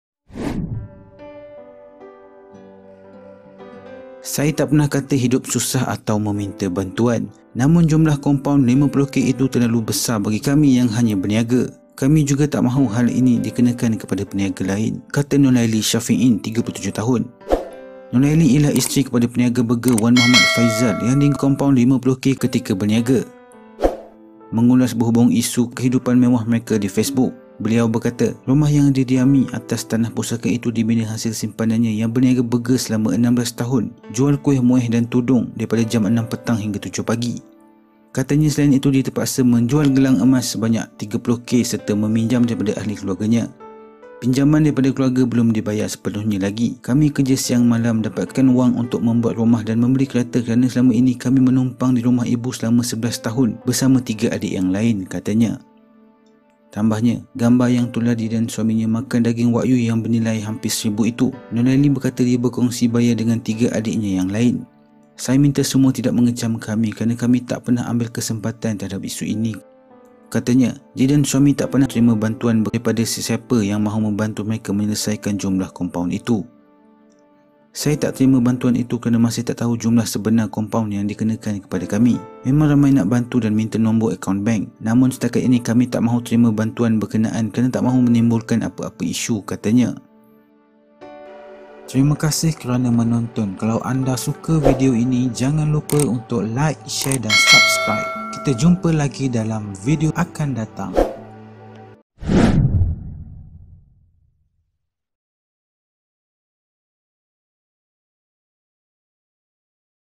Peniaga Burger RM50K Sebenarnya Hidup Mewah? Isteri Tampil Bagi Penjelasan Selepas Teruk DIKECAM
Peniaga Burger Dikompaun RM50K Sebak Dedah Apa Sebenarnya Terjadi Dakwaan Hidup Mewah, Tipu Netizen

Peniaga Faisal Burger DIKOMPAUN RM50K Sebenarnya Hidup Mewah? Ada Rumah Besar, Kereta Mewah, Makan Wagyu RM1000? Isteri Tampil Bagi Penjelasan Selepas Teruk DIKECAM Buat Ramai TAK SANGKA TERKEJUT